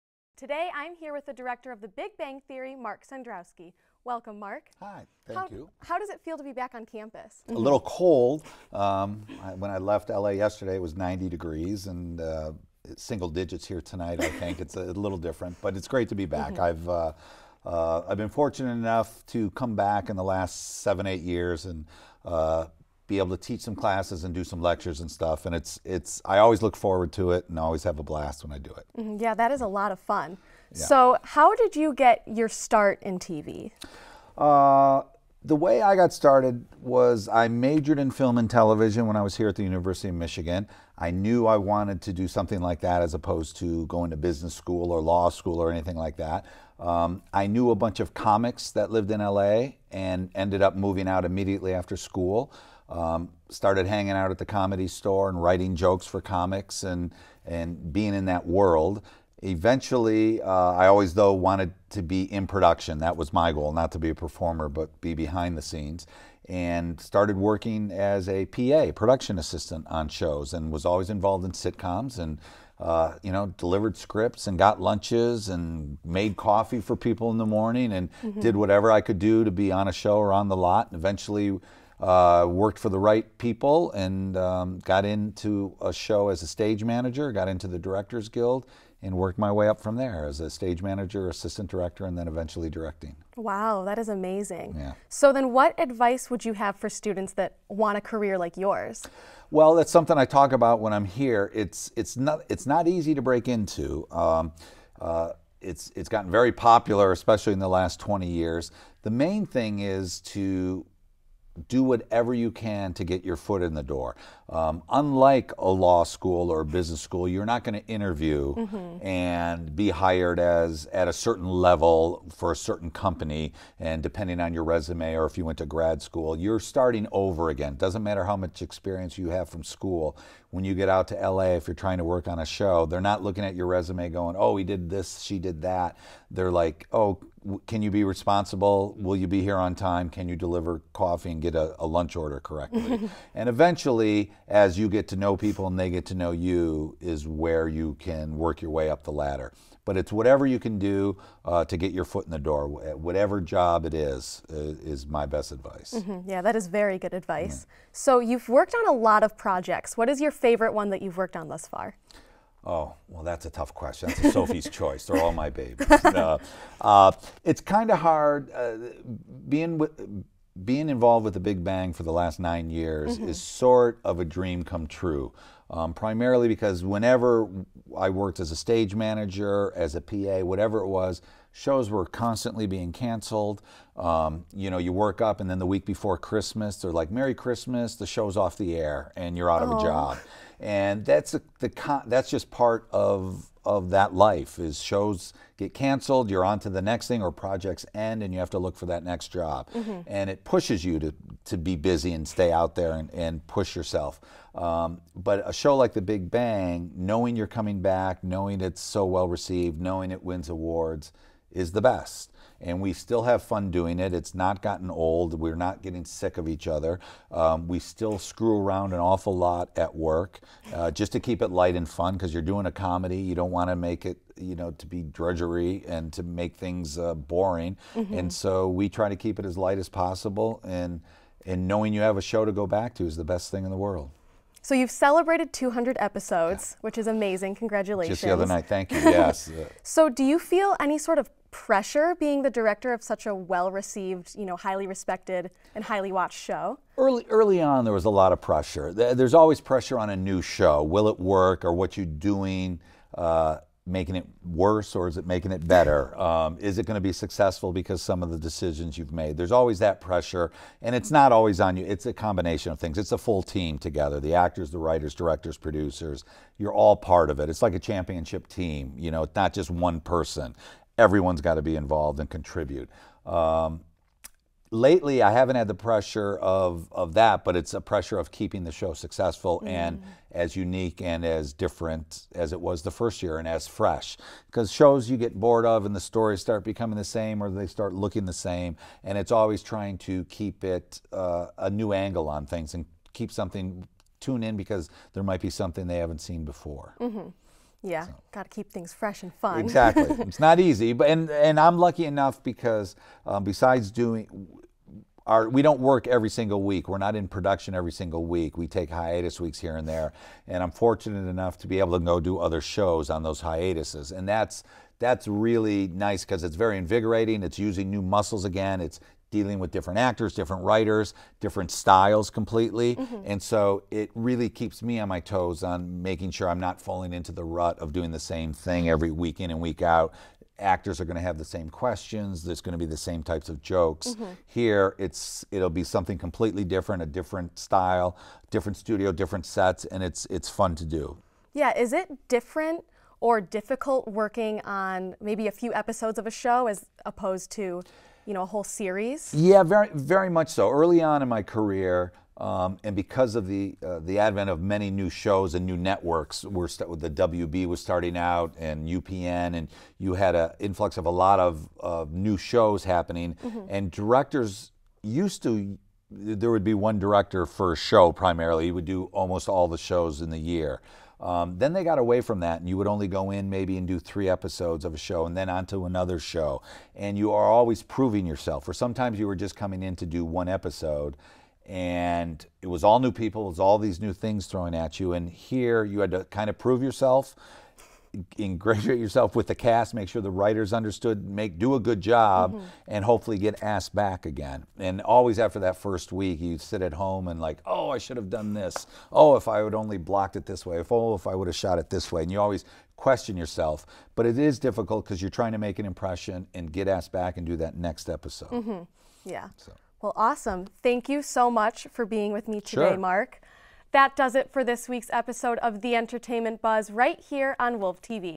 Interview with Mark Cendrowski | The Entertainment Buzz
The Entertainment Buzz's Jess Knight interviews The Big Bang Theory director Mark Cendrowski. They discuss advice about getting into the industry, the pressure of being the director of TV's most watched comedy, and his favorite projects to date.

Special thanks to Mark Cendrowski and Terri Sarris for the opportunity for this interview.